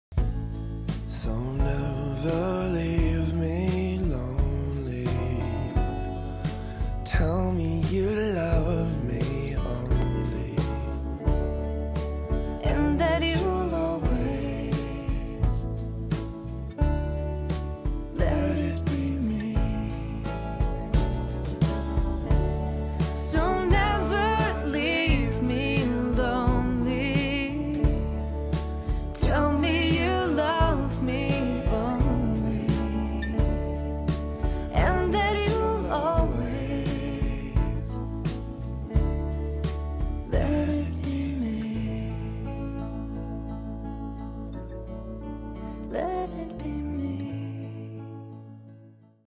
Ensaio Pre-Wedding Mayara e Flávio
Filmagem por Erik Marreiro.
Realizado na Casa de Campo (Itambé - PE), em 19 de maio de 2016.